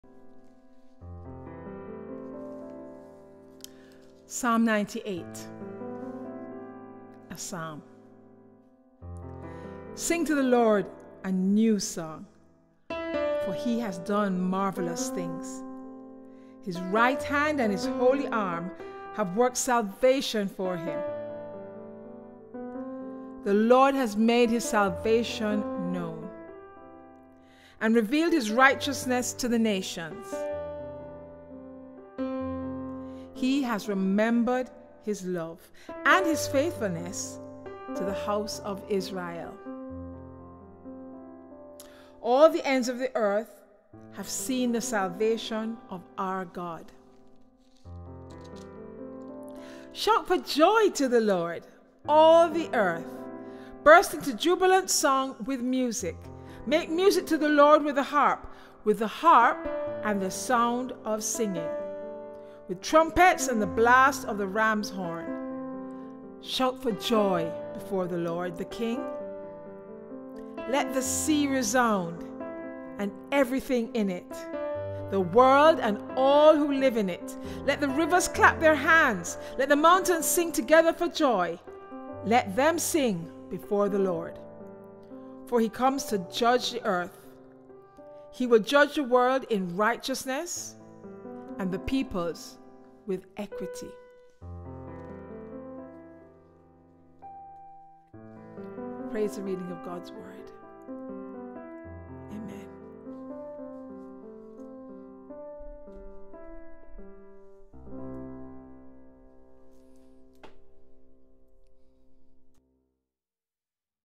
Online Worship - Psalm 98 (NIV version) read by Thea Wray .
A Psalm of praise …….
Music composed and played by Thea Wray - August 2020